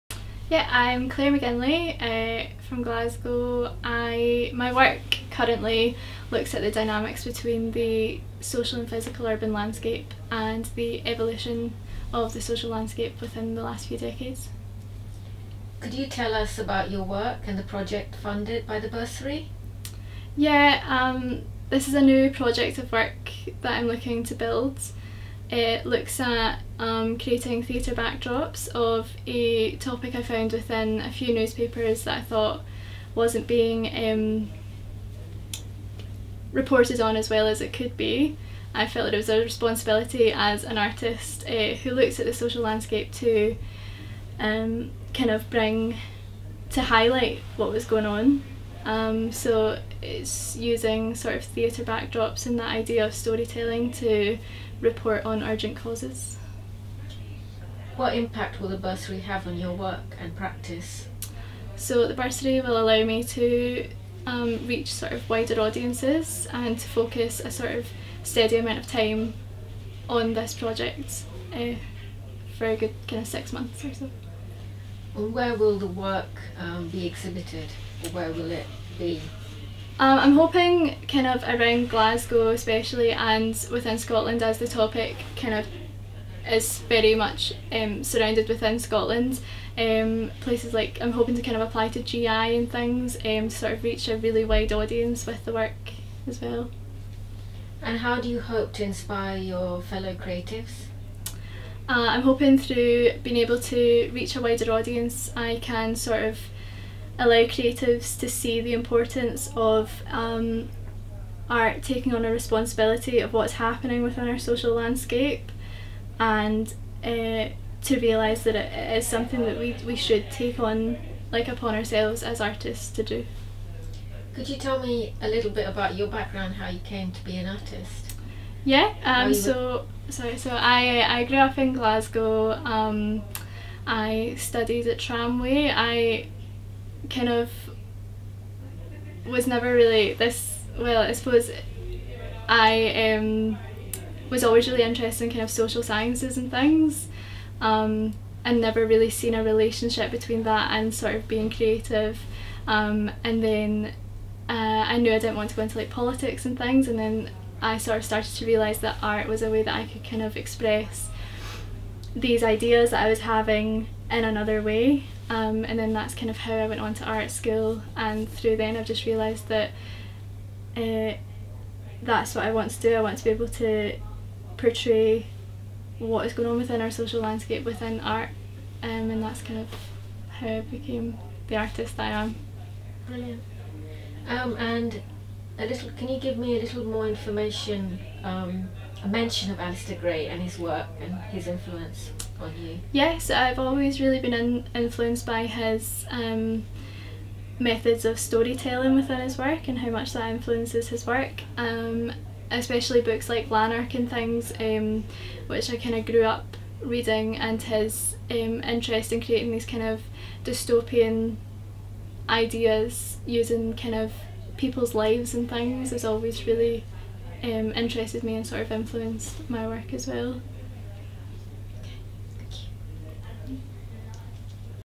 Claire McGinlay - winner of the 2019 Alasdair Gray Inspiring Scotland Bursary
The winner of The Alasdair Gray Inspiring Scotland Bursary is artist Claire McGinlay whose proposed body of paintings will focus on the whitewashing of abuse claims at the Sisters of Nazareth Orphanage in the 1970s and 80s.

Claire says: I am thrilled to have won this award. It reassures my confidence in the work I am currently developing that explores the impact of story-telling to highlight urgent causes. I hope to reach a wider audience and highlight the evolution of our social landscape in relation to art practice.

The Inspiring Scotland Bursaries were created to celebrate emerging creatives, based in Scotland, who demonstrate exceptional potential and whose development will be greatly benefitted by the funds. The Alasdair Gray Inspiring Scotland Bursary, first created in 2017, is open annually to post-secondary students pursuing a career in music, fine arts, theatre, dance or cultural studies and seeks to fund innovative, multi-disciplinary creative proposals.